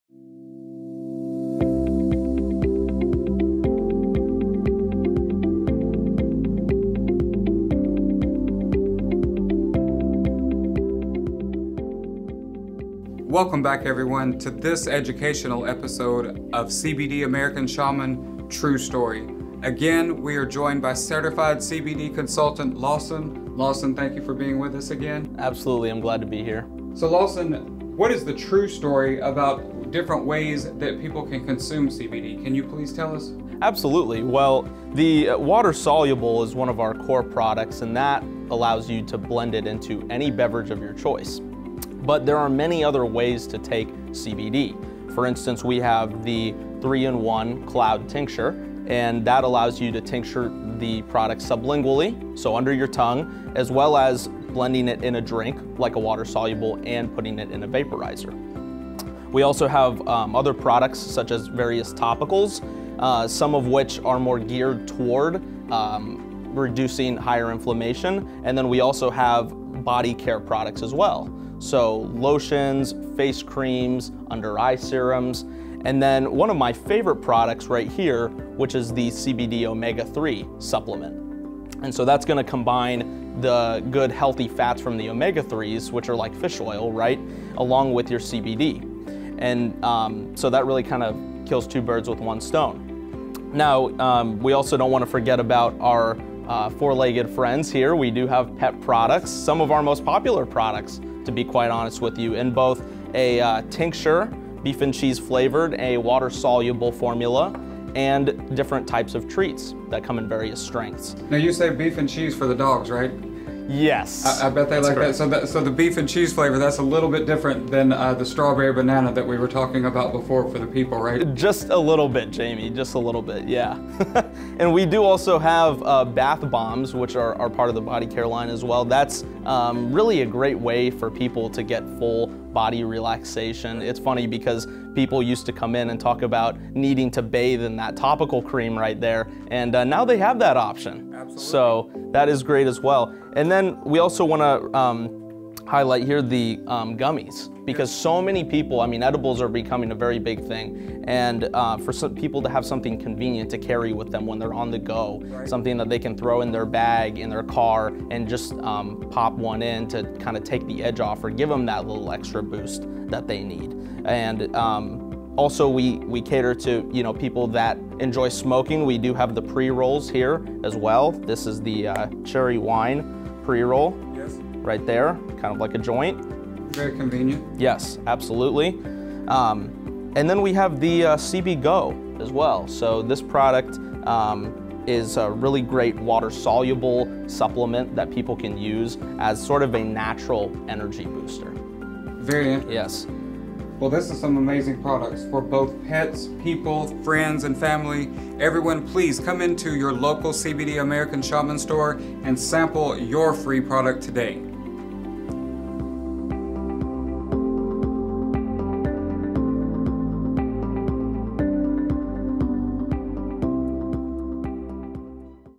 True Story of American Shaman CBD Series Ep. 8: How Do I Consume CBD?
Interested in learning what types of CBD products are available? There are many ways to administer CBD and in this video, Certified CBD Consultant Lawson Kalaiwaa, explains how CBD be used in various ways. Check it out!